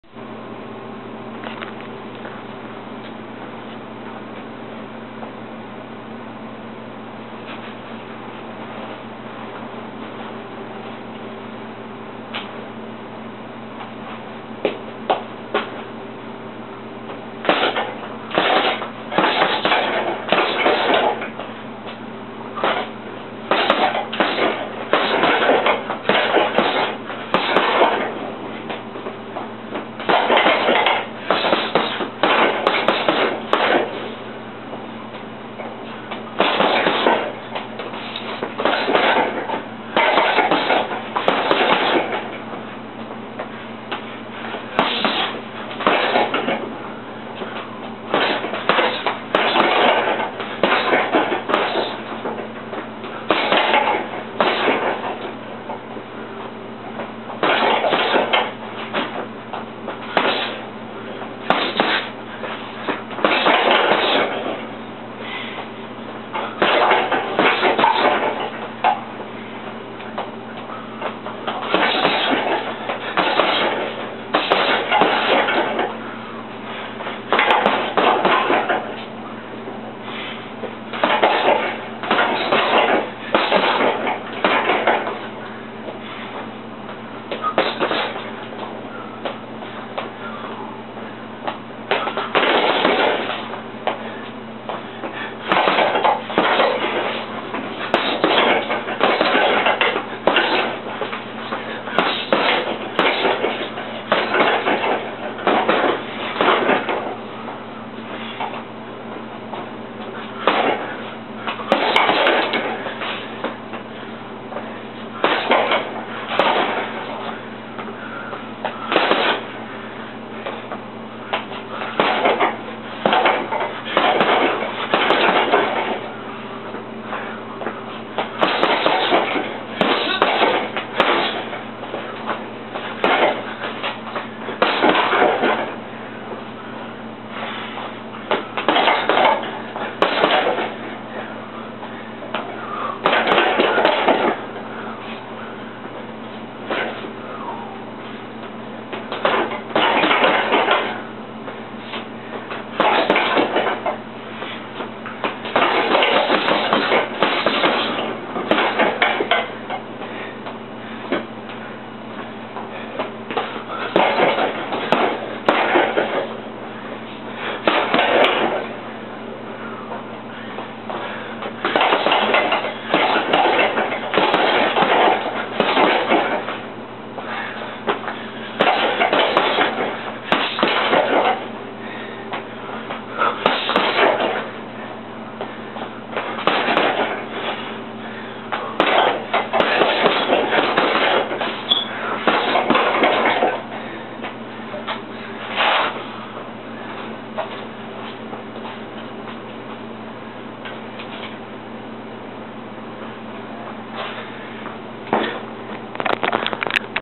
Hitting Heavy Bag
Just working the bag after doing some roadwork